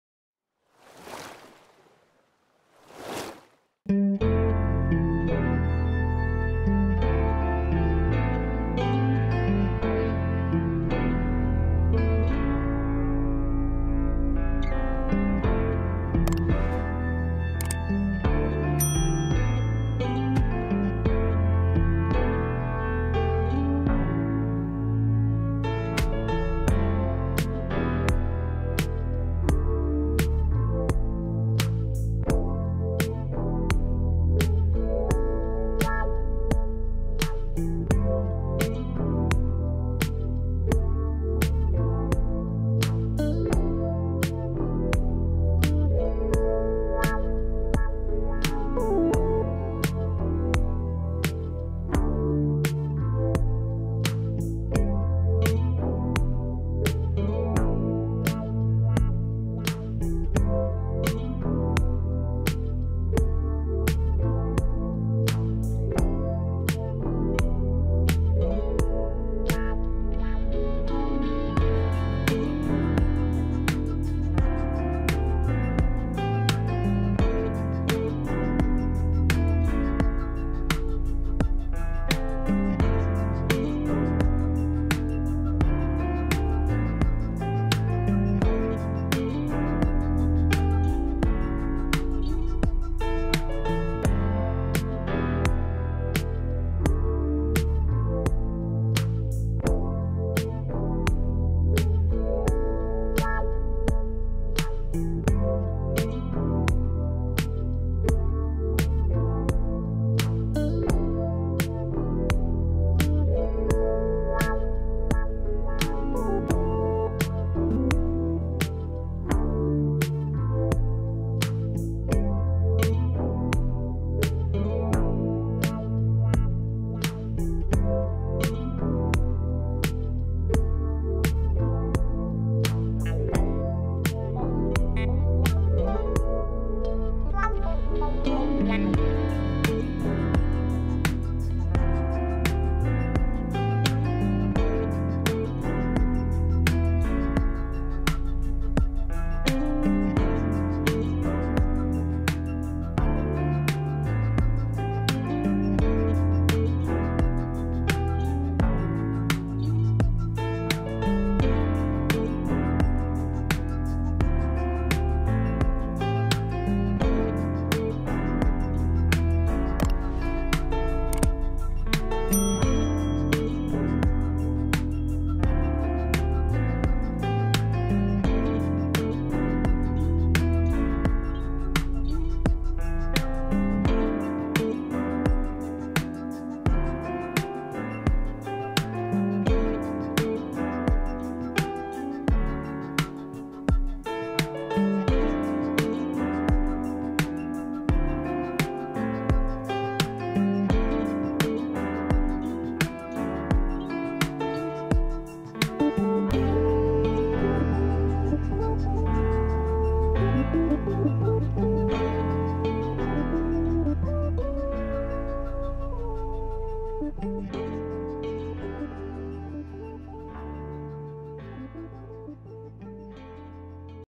Jewelry under the MICROSCOPE // invisible world
In this video you will see how Jewelry look under a microscope. Let's look into the microworld!